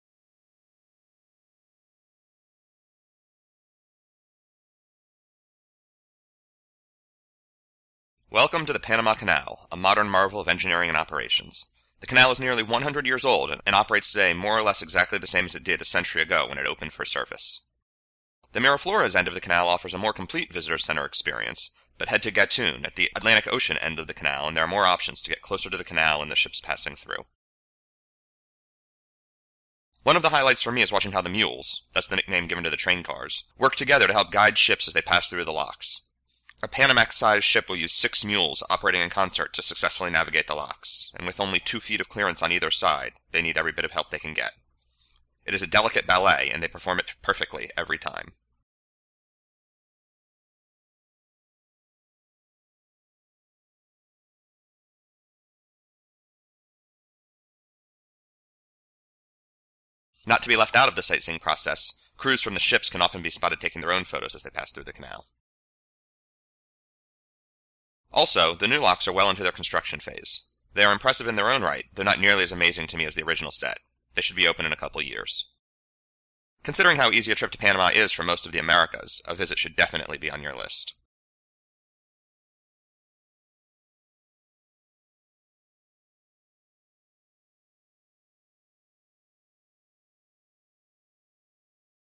A visit to the Panama Canal
Welcome to the Panama Canal. It has been in operation for nearly 100 years and it still is amazing to me. This time around (my 4th visit) I finally got to see the Gatun Locks operation at the Atlantic Ocean end of the Canal, near Colon, Panama. While a smaller visitors center and without the museum and video, the experience is much, much more intimate. You are quite a bit closer to the ships and crowds do not exist. I suppose that's the trade-off for it being a 90 minutes further drive from Panama City.

Read more about the visit on the Wandering Aramean Blog